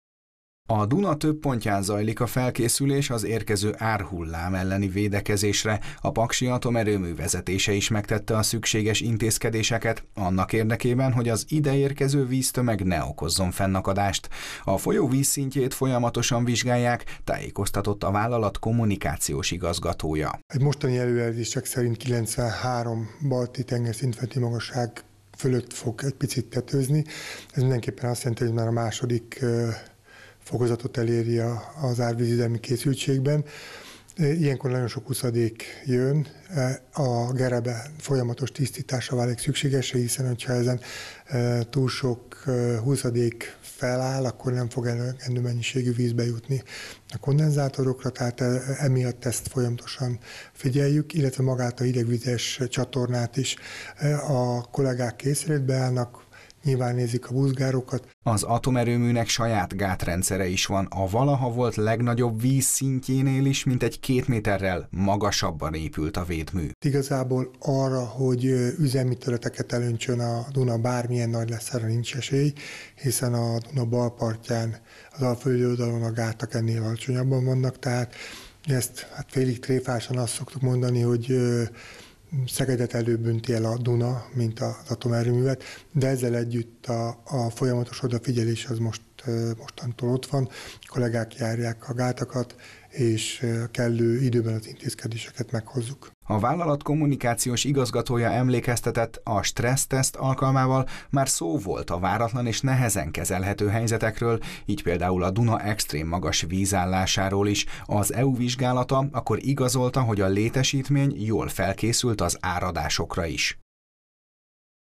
Folyamatosan monitorozzák a Duna vízszintjét az atomerőműben is
A valaha volt legnagyobb vízállásnál is két méterrel magasabban van a paksi atomerőmű saját gátja, így szinte kizárt, hogy a mostani árhullám üzemi területeket érintsen – erről tájékoztatott a vállalat kommunikációs igazgatója. Dr. Kovács Antal szerint a létesítményben felkészültek az érkező áradásra, a vízszint változását folyamatosan monitorozzák.